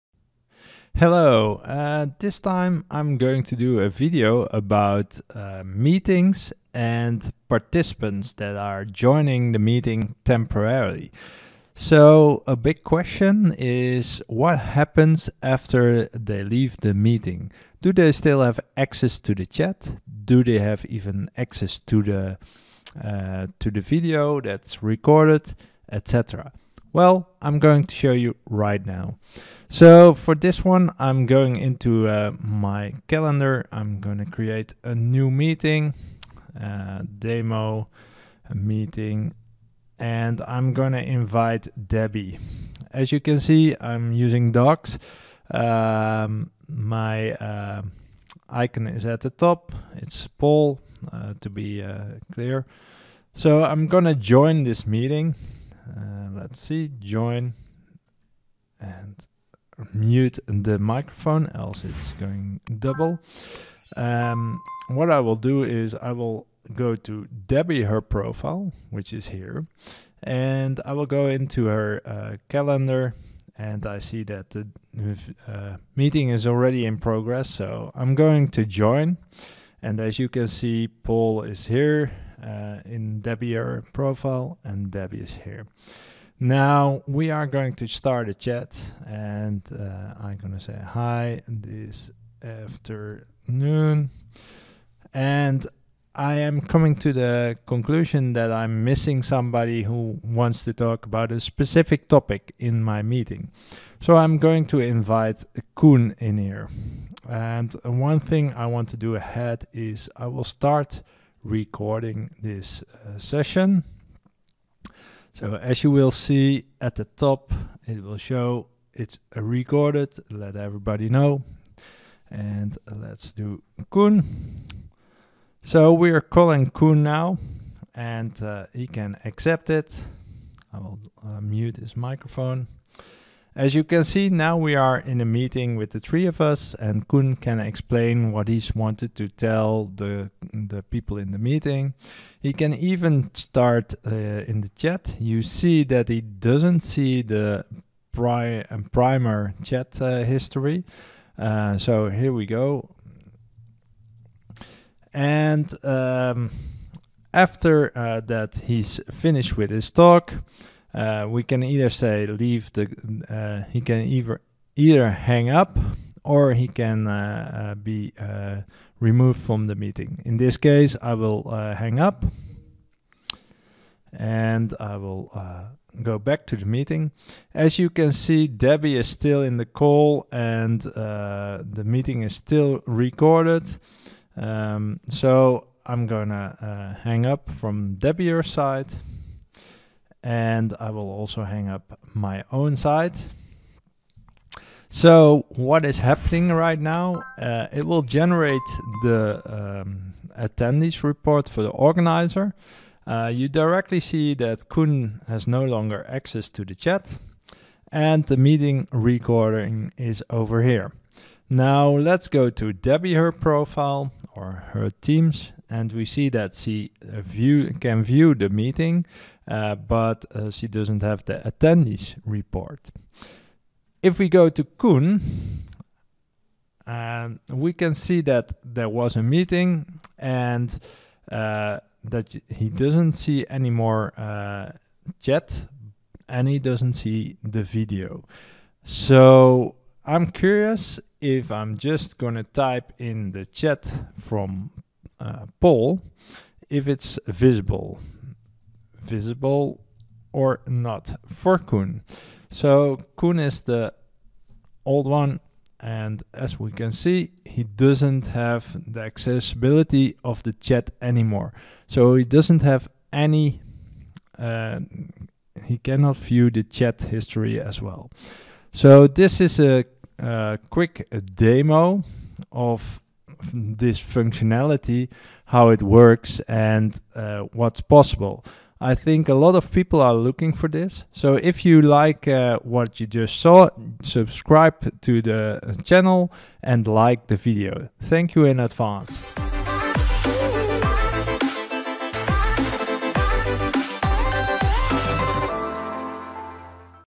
Temporary guests in teams meeting
In this video I will explain what happens when you add guests to your meeting. And what they can do with the chat and video. Please watch the video to get all insights.

If you want to know more:
Follow Paul Keijzers on LinkedIn or Twitter of checkout the website KbWorks.nl

0:00 start
0:31 demo of meeting
4:30 Outro